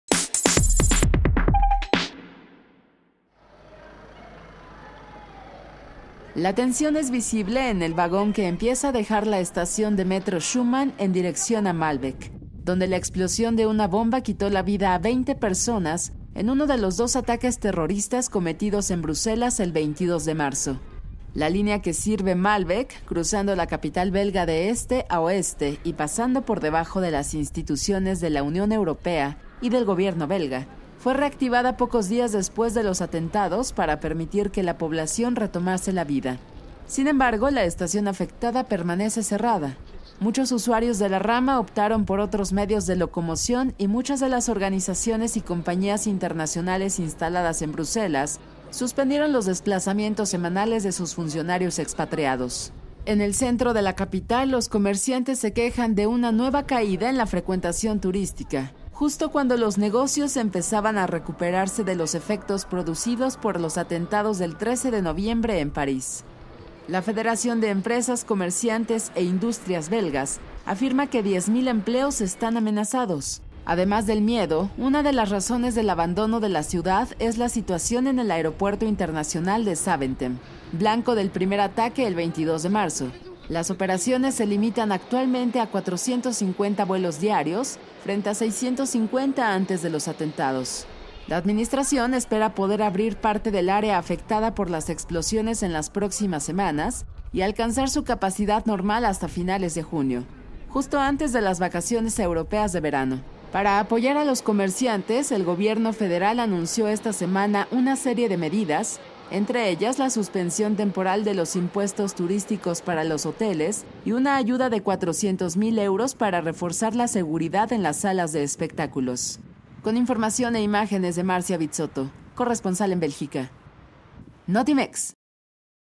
Un mes después de atentados, Bruselas pena para recobrar vida
Tras los atentados del 22 de marzo, el aeropuerto internacional de Zaventem limita los vuelos de 650 a 450 por día